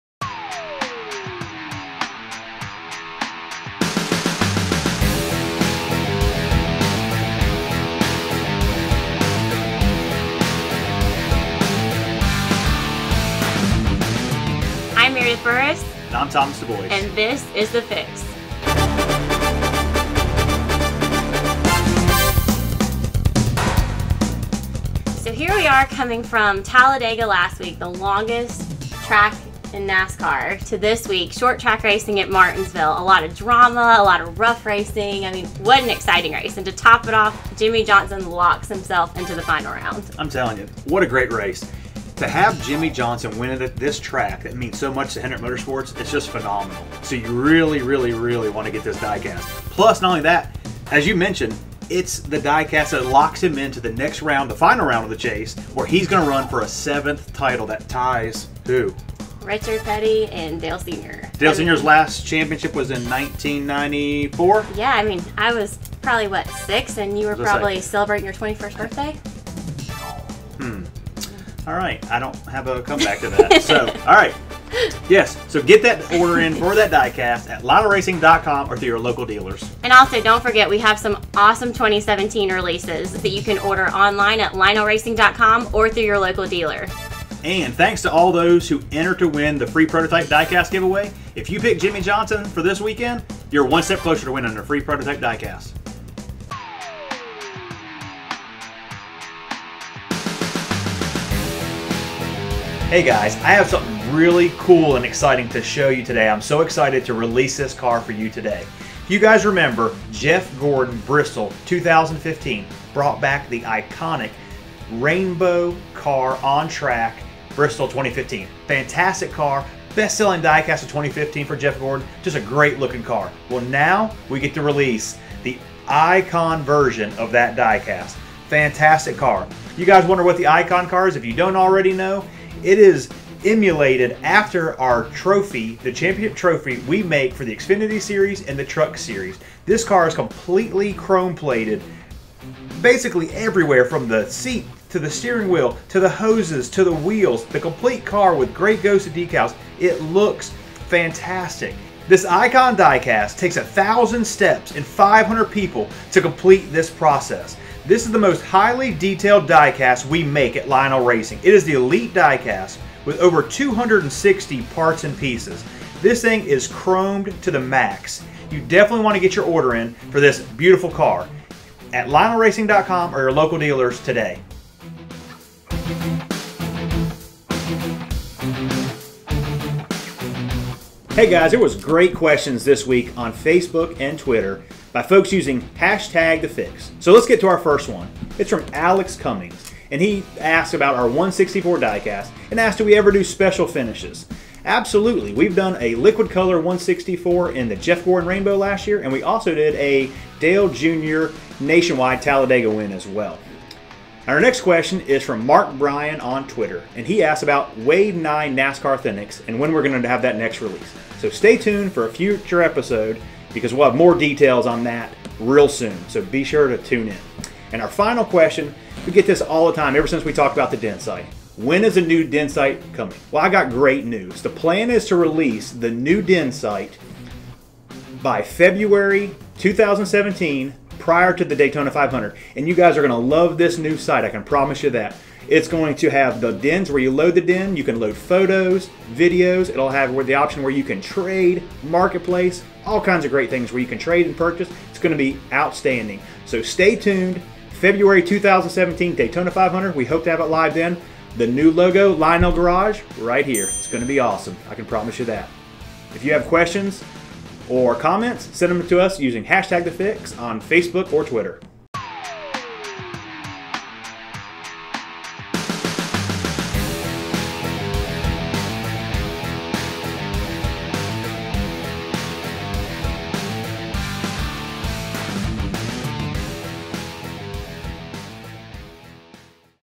"The Fix" - Jeff Gordon Prototype Preview
Jimmie Johnson is locked in to contend for his seventh NASCAR Sprint Cup Series championship and we're taking a first look at Jeff Gordon's Axalta Rainbow ICON prototype. Plus, we've got good news about our all new DIN registration site!